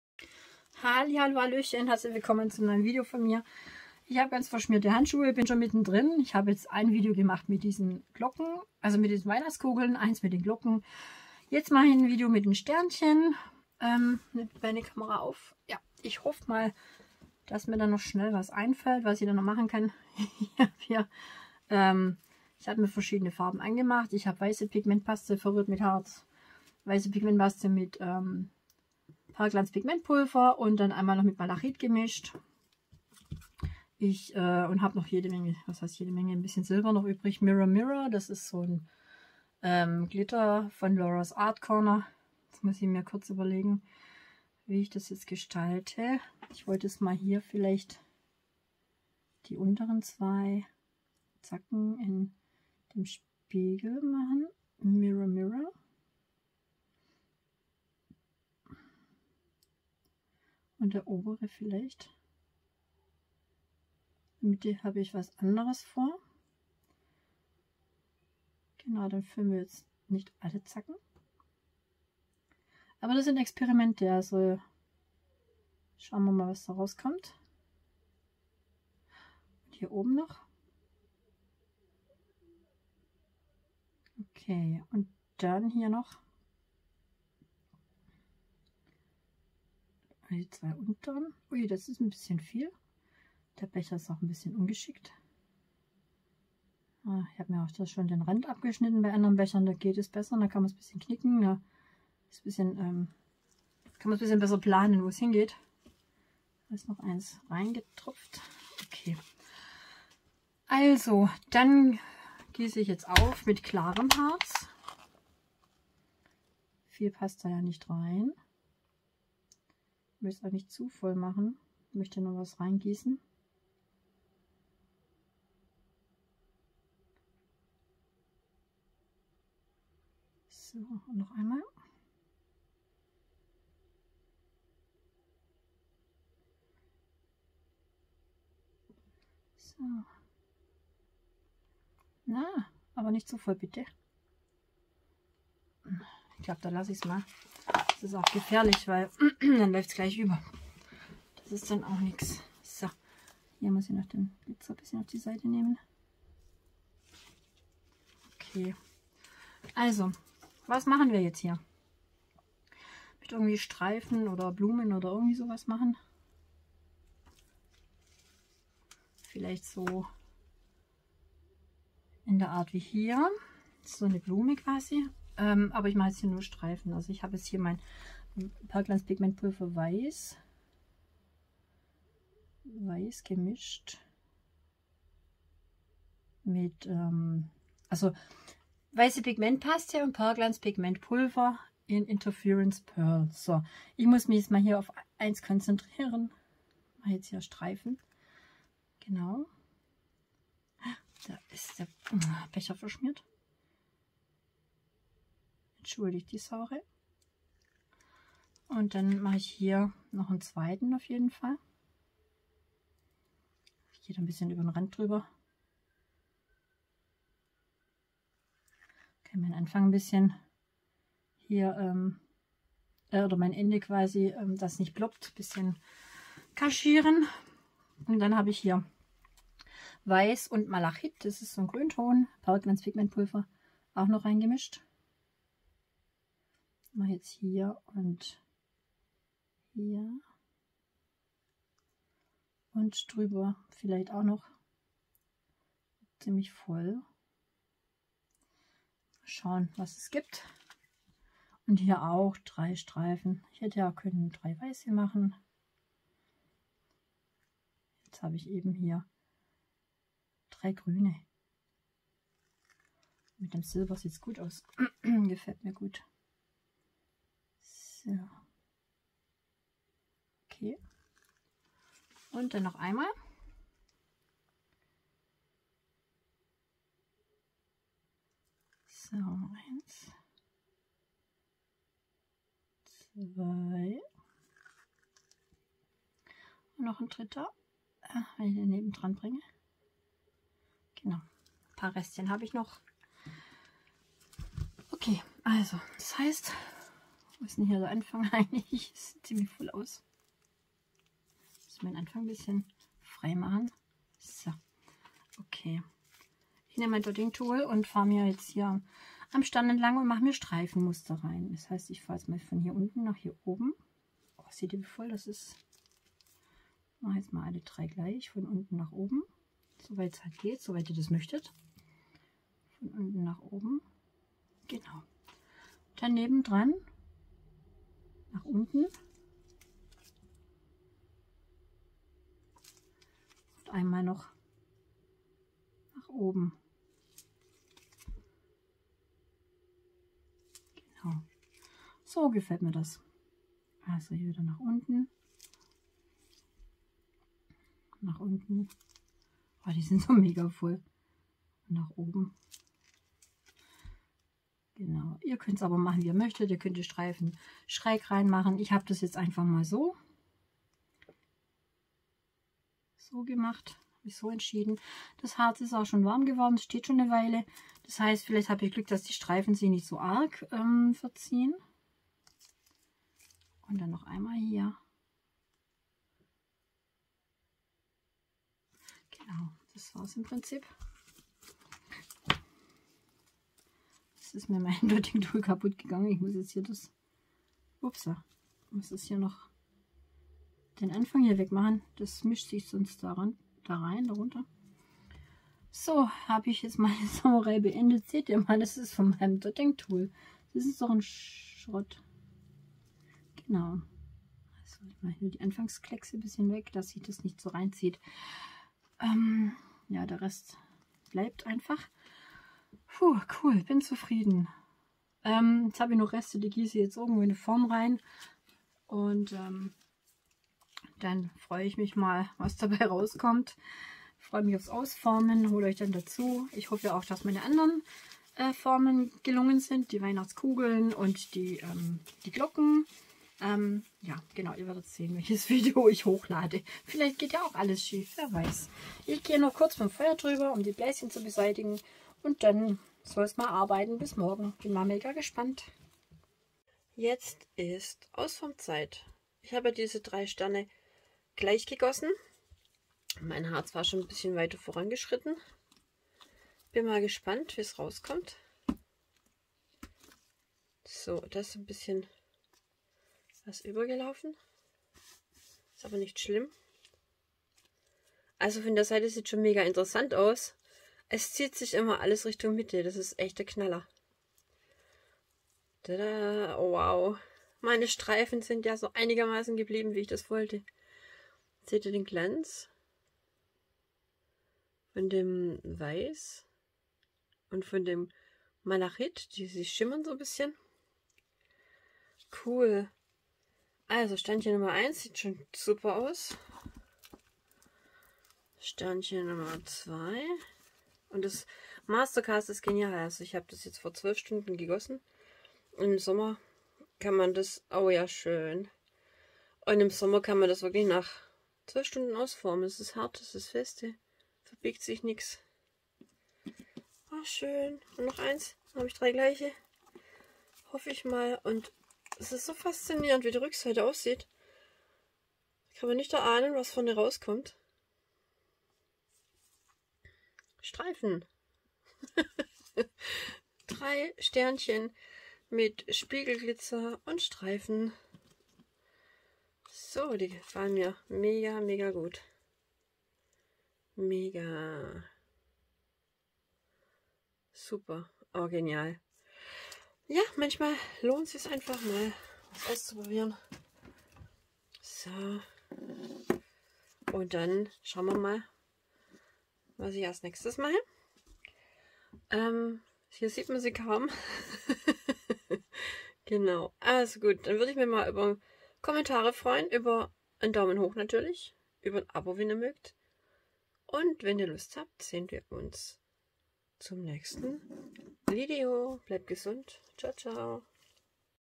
#283 Epoxidharz Weihnachtssterne ✨️ Resin xmas stars ✨️
Es weihnachtet ✨️

Gegossen mit dem Mastercast, der weissen Pigmentpaste, dem Perlglanz Pigmentpulver "Interference Pearl" und "Malachit" von @etter_art.

Die Silikonform ist von @moldsandshapes 🥰

Der Glitter ist von @laurasartcorner250 🥰